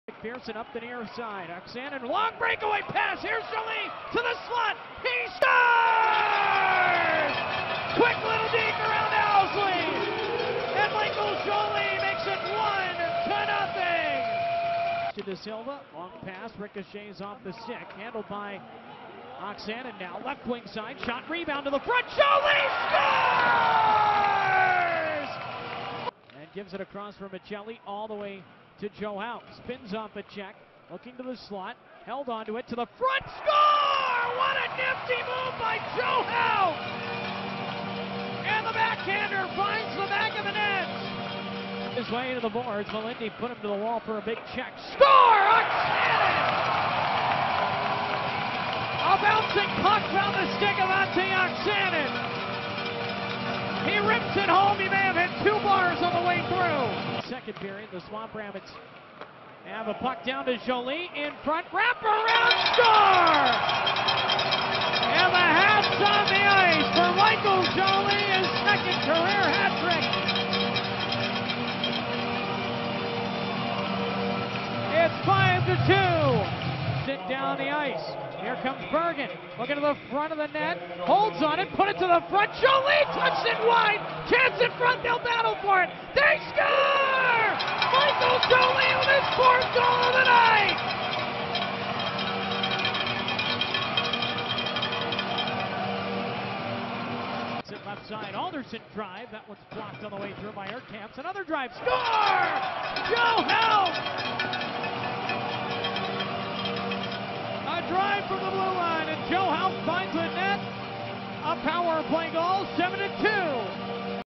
Game Highlights | 3-07-17
The Greenville Swamp Rabbits (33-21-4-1), proud ECHL affiliate of the New York Rangers and the AHL’s Hartford Wolf Pack, rode the backs of Michael Joly, Ahti Oksanen, and Allan McPherson to a 7-3 win against the Wichita Thunder (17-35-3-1). The Swamp Rabbits statement win began a stretch of five games in six days. Joly, Oksanen, and McPherson each earned career highs, each earning four points in the victory. Joly scored 4 goals in 2 periods to give him his 2nd career hat trick!